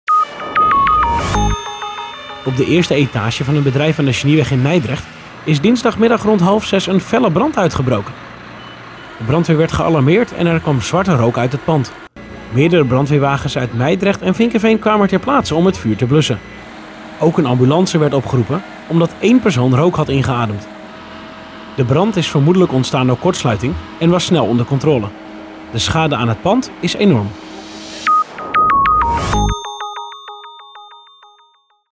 Korte felle brand op bedrijventerrein Mijdrecht - 0297.nl
De brandweer werd rond 17:25 uur opgeroepen omdat er zwarte rook uit het pand kwam. Meerdere brandweerwagens uit Mijdrecht en Vinkeveen kwamen ter plaatse om het vuur te blussen.